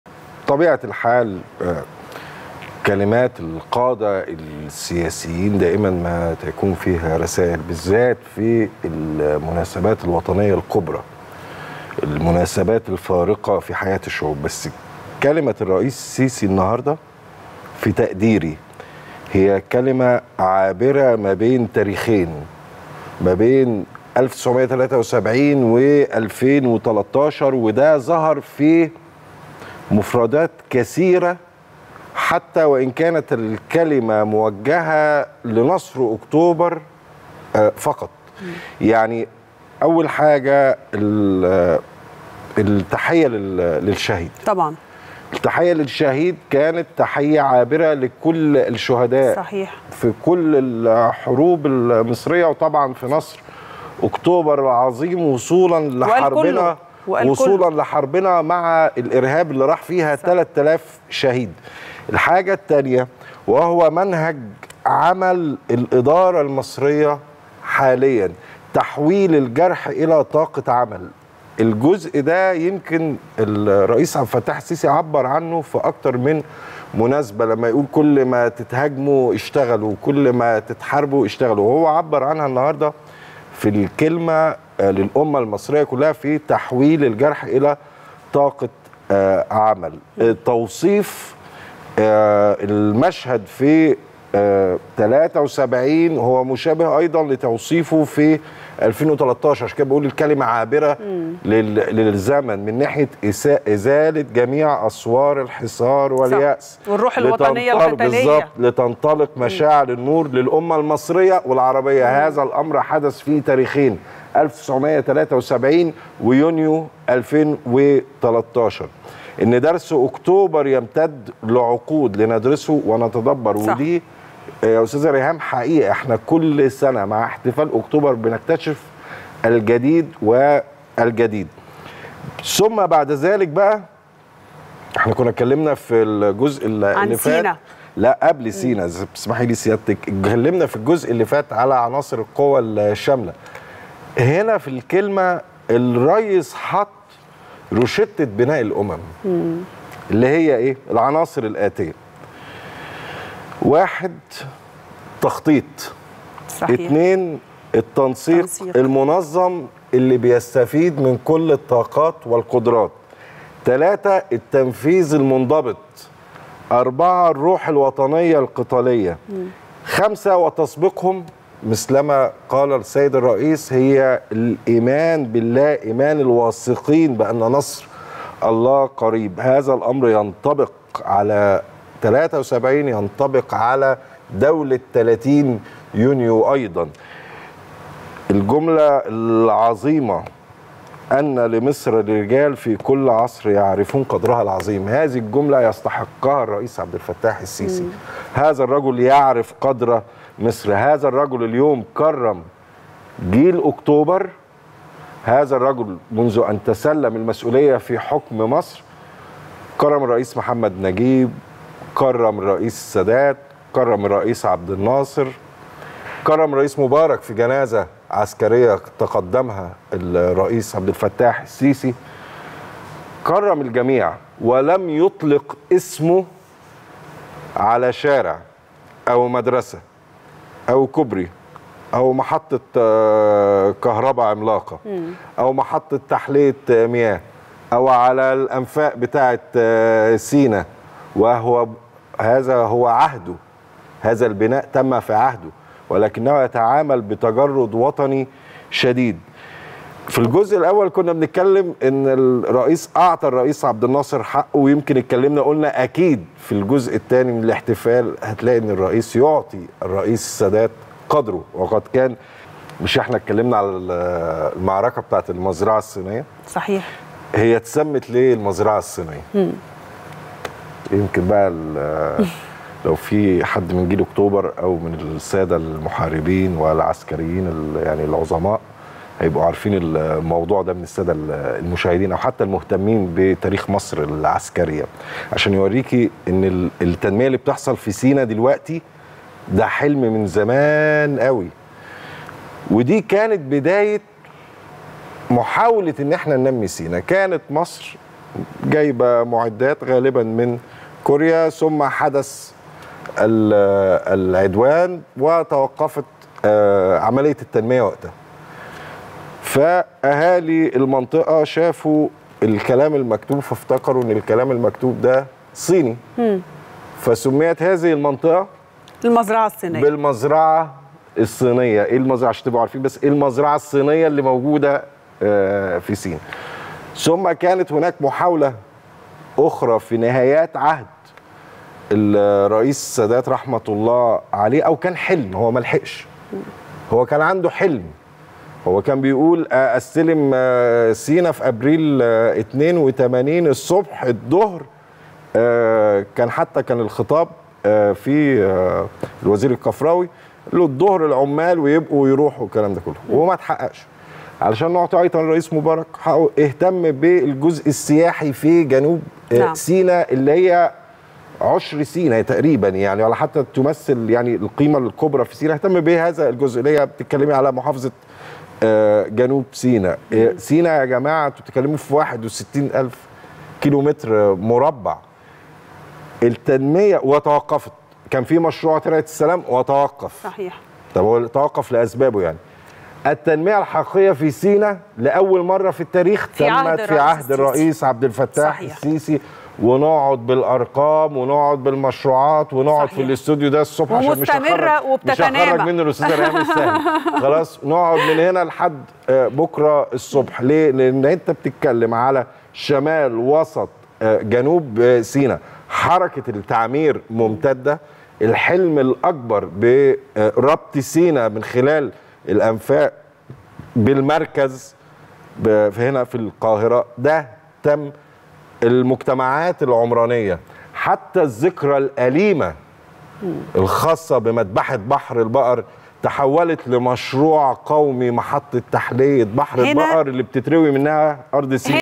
"وضع روشتة بناء الأمم".. أحمد الطاهري يُبرز رسائل الرئيس السيسي خلال كلمته في احتفال نصر أكتوبر
"وضع روشتة بناء الأمم"..
أحمد الطاهري يُبرز رسائل الرئيس السيسي خلال كلمته في احتفال نصر أكتوبر

4 أكتوبر 2023